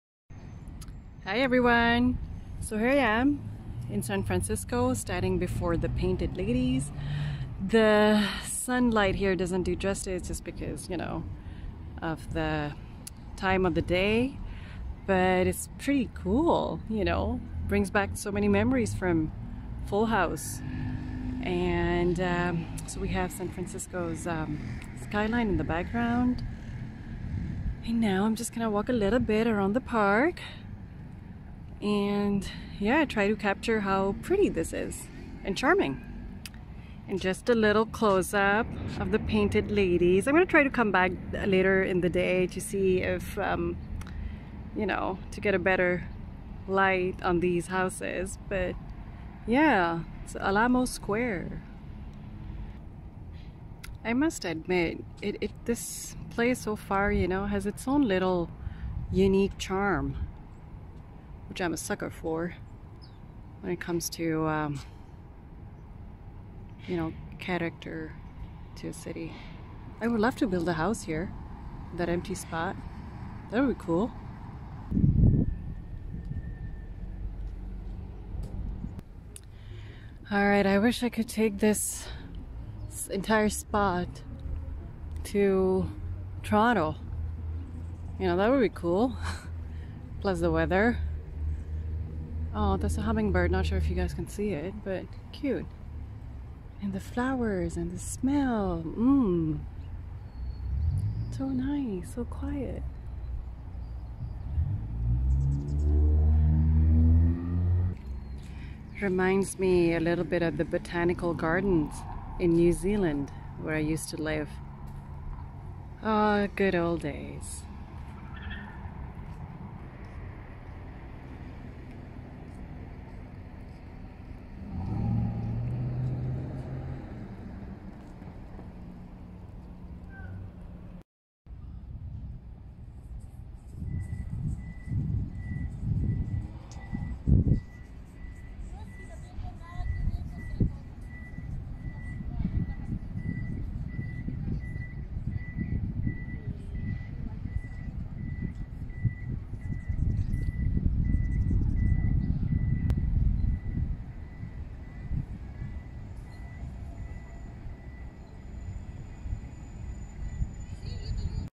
Randomness #32: Painted Ladies
Full House! I watched the series growing up and the layout always used to attract me.
It's a beautiful little spot to hangout with friends or by yourself, read a book, picnic or even a romantic date :)